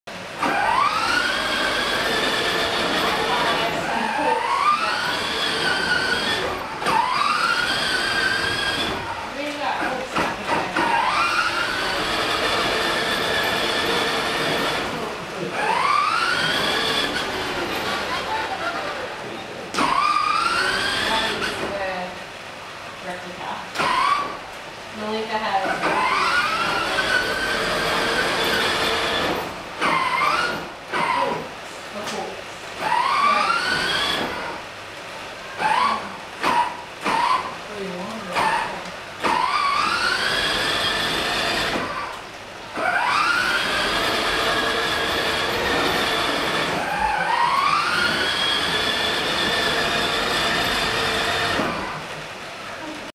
This is why you need a back yard!
I found this footage of my niece on an electric quad bike really funny.